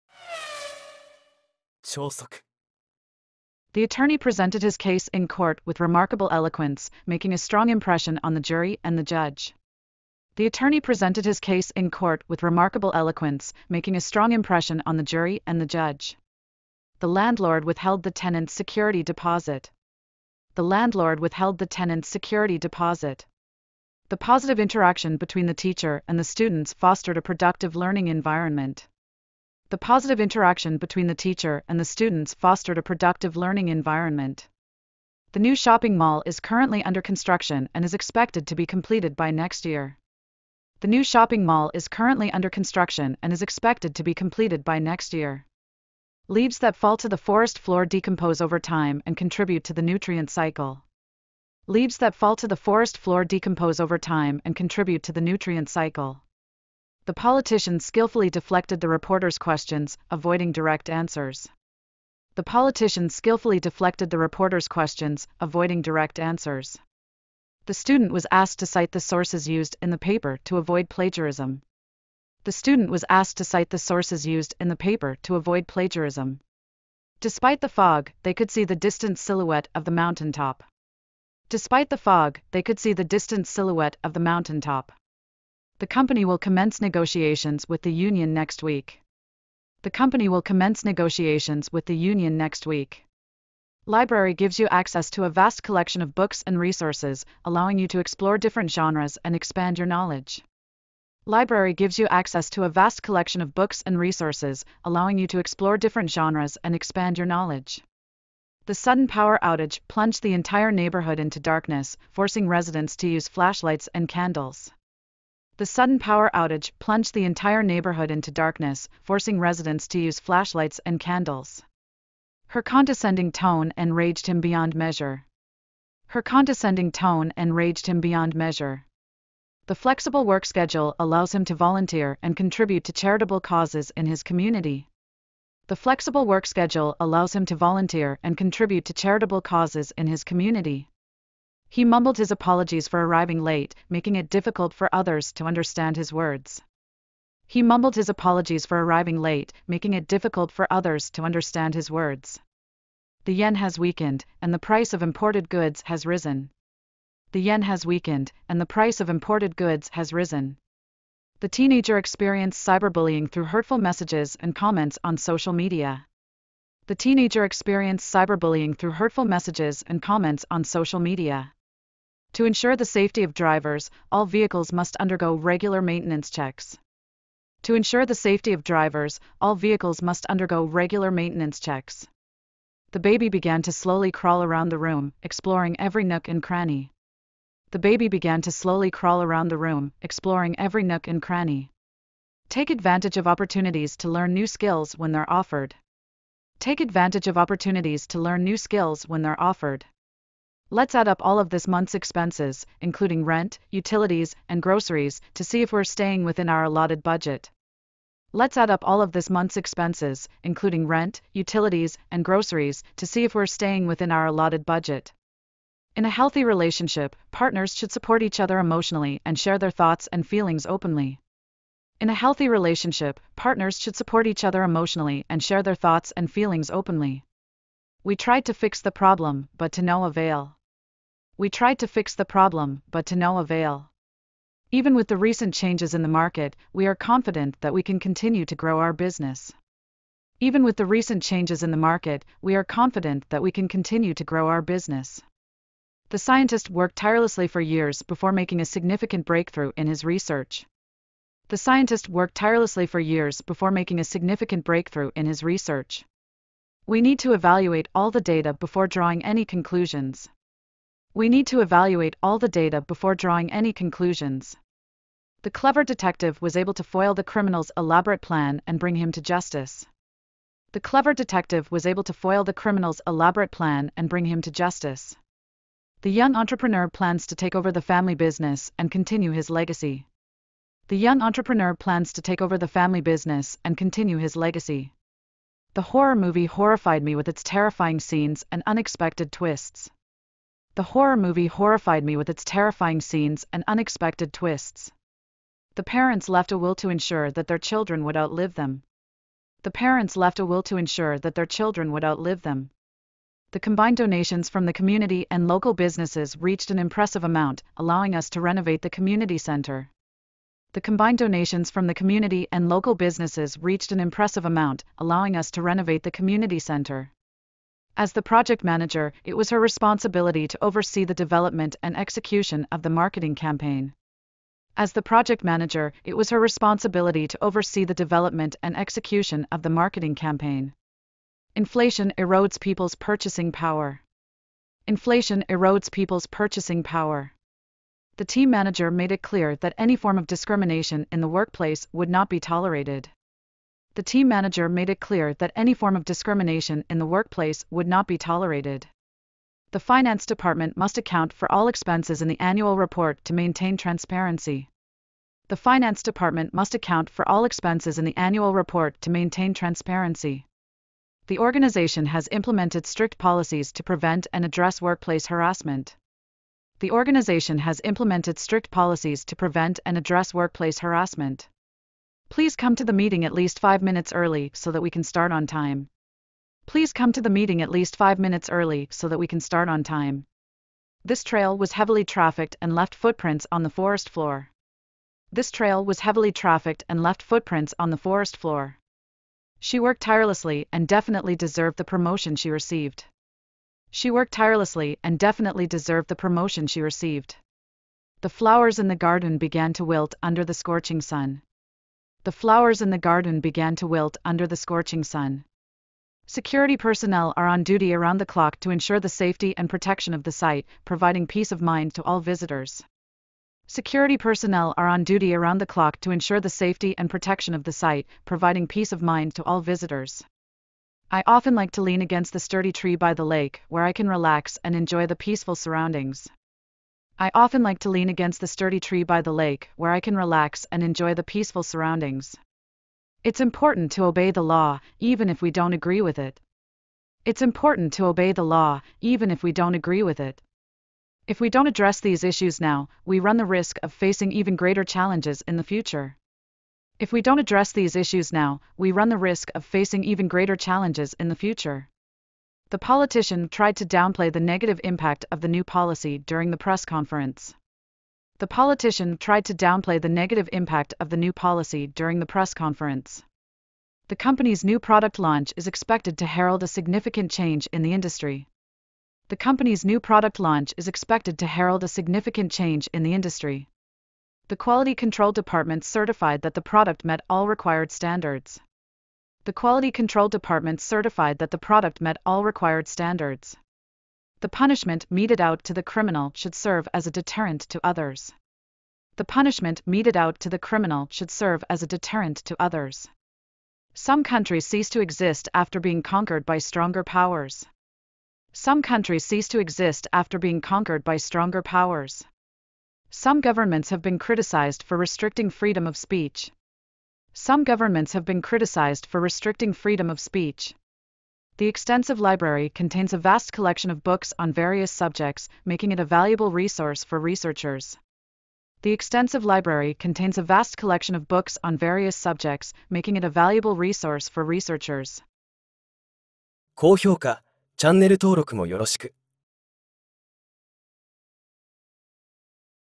231202-03【例文音読・土日は復習】超速英語リスニング
土日は今週の分を繰り返し復習しましょう。
今週アップした英文を話者を変えて5分にまとめました。

The attorney presented his case in court with remarkable eloquence, making a strong impression on the jury and the judge.
The landlord withheld the tenant's security deposit.
The positive interaction between the teacher and the students fostered a productive learning environment.
The new shopping mall is currently under construction and is expected to be completed by next year.
Leaves that fall to the forest floor decompose over time and contribute to the nutrient cycle.
The politician skillfully deflected the reporter's questions, avoiding direct answers.
The student was asked to cite the sources used in the paper to avoid plagiarism.
Despite the fog, they could see the distant silhouette of the mountaintop.
The company will commence negotiations with the union next week.
Library gives you access to a vast collection of books and resources, allowing you to explore different genres and expand your knowledge.
The sudden power outage plunged the entire neighborhood into darkness, forcing residents to use flashlights and candles.
Her condescending tone enraged him beyond measure.
The flexible work schedule allows him to volunteer and contribute to charitable causes in his community.
He mumbled his apologies for arriving late, making it difficult for others to understand his words.
The yen has weakened, and the price of imported goods has risen.
The teenager experienced cyberbullying through hurtful messages and comments on social media.
To ensure the safety of drivers, all vehicles must undergo regular maintenance checks.
The baby began to slowly crawl around the room, exploring every nook and cranny.
Take advantage of opportunities to learn new skills when they're offered.
Let's add up all of this month's expenses, including rent, utilities, and groceries, to see if we're staying within our allotted budget.
In a healthy relationship, partners should support each other emotionally and share their thoughts and feelings openly.
We tried to fix the problem, but to no avail.
Even with the recent changes in the market, we are confident that we can continue to grow our business.
The scientist worked tirelessly for years before making a significant breakthrough in his research.
We need to evaluate all the data before drawing any conclusions.
The clever detective was able to foil the criminal's elaborate plan and bring him to justice.
The young entrepreneur plans to take over the family business and continue his legacy.
The horror movie horrified me with its terrifying scenes and unexpected twists.
The parents left a will to ensure that their children would outlive them.
The combined donations from the community and local businesses reached an impressive amount, allowing us to renovate the community center.
As the project manager, it was her responsibility to oversee the development and execution of the marketing campaign.
Inflation erodes people's purchasing power.
The team manager made it clear that any form of discrimination in the workplace would not be tolerated.
The finance department must account for all expenses in the annual report to maintain transparency.
The organization has implemented strict policies to prevent and address workplace harassment.
Please come to the meeting at least 5 minutes early so that we can start on time.
This trail was heavily trafficked and left footprints on the forest floor.
She worked tirelessly and definitely deserved the promotion she received.
The flowers in the garden began to wilt under the scorching sun.
Security personnel are on duty around the clock to ensure the safety and protection of the site, providing peace of mind to all visitors.
I often like to lean against the sturdy tree by the lake, where I can relax and enjoy the peaceful surroundings.
It's important to obey the law, even if we don't agree with it.
If we don't address these issues now, we run the risk of facing even greater challenges in the future.
The politician tried to downplay the negative impact of the new policy during the press conference.
The company's new product launch is expected to herald a significant change in the industry.
The quality control department certified that the product met all required standards.
The punishment meted out to the criminal should serve as a deterrent to others.
Some countries ceased to exist after being conquered by stronger powers.
Some governments have been criticized for restricting freedom of speech.
The extensive library contains a vast collection of books on various subjects, making it a valuable resource for researchers.